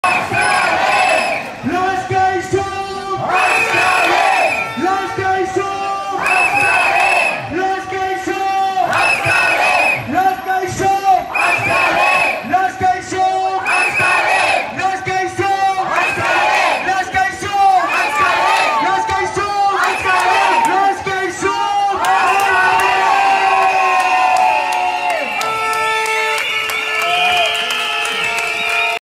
Docentes interinos catalanes piden mejoras y parar las oposiciones
Los manifestantes bajan por Via Laietana en dirección a plaza Sant Jaume para reivindicar sus peticiones